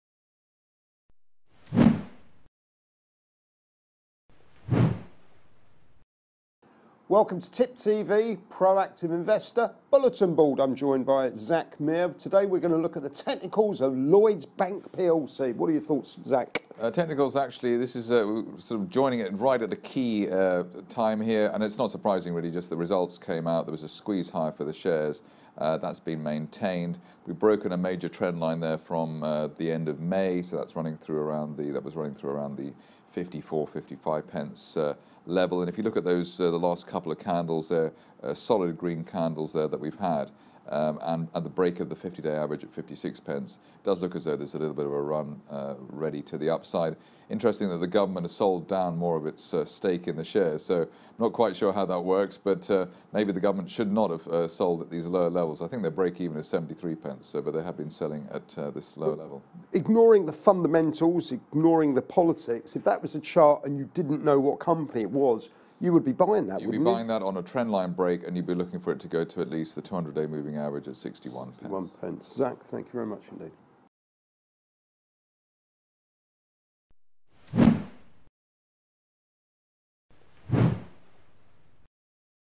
Lloyds Banking Group shares are making a run higher, chartist sees a rise to 61p
Lloyds Banking Group shares are making a run higher, according to chartist Zak Mir, who reckons the price will soon rise to 61p.
Mir, in a Tip TV segment for Proactive Investors, notes that the bank’s shares have squeezed higher and have hit a number of significant technical markers along the way.
In fact, he says this is a particularly key moment for the Lloyds chart. He added: “[the shares are] at the break of the 50-day moving average at 56p, it does look as though there’s a little bit of a run to the upside.”